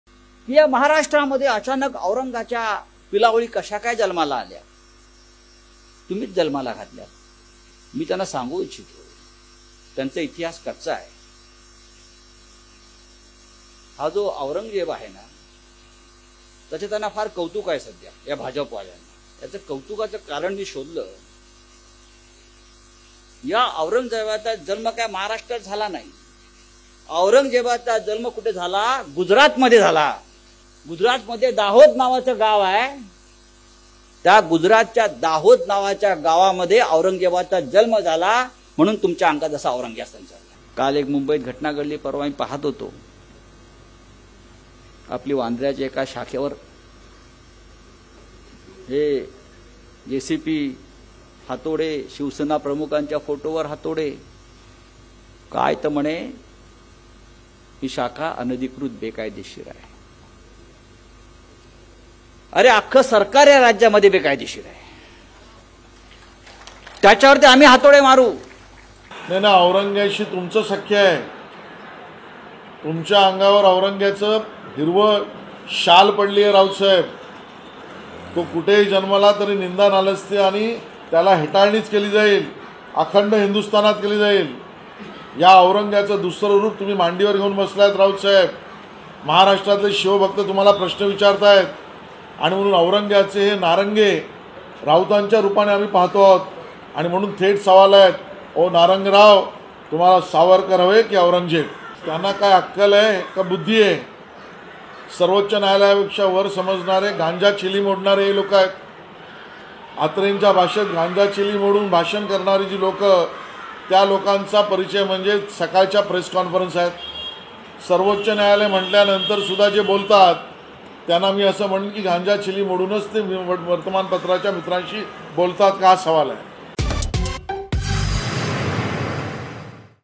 औरंग्याचे नारंगे राऊतांच्या रूपात पाहतोय - आशिष शेलार | Ashish Shelar | Sanjay Raut
Ashish Shelar on Sanjay raut,Ashish Shelar on Uddhav Thackeray,Ashish Shelar on Auranzeb,Uddhav Thackeray,Devendra fadnavis,BJP,Ashish Shelar,Sanjay Raut,